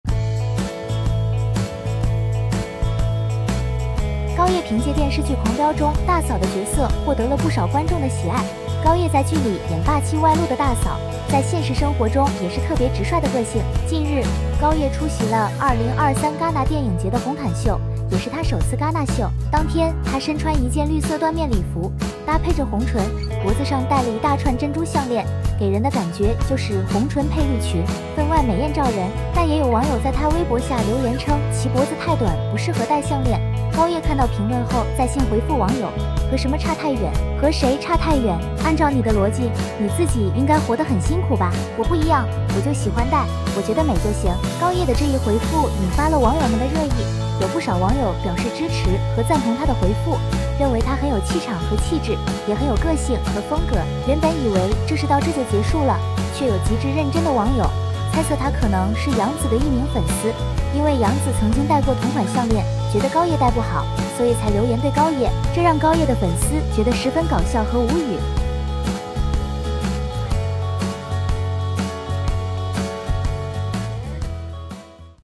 “大嫂”高葉回懟網友吐槽脖子短 被深挖出其疑似楊紫粉絲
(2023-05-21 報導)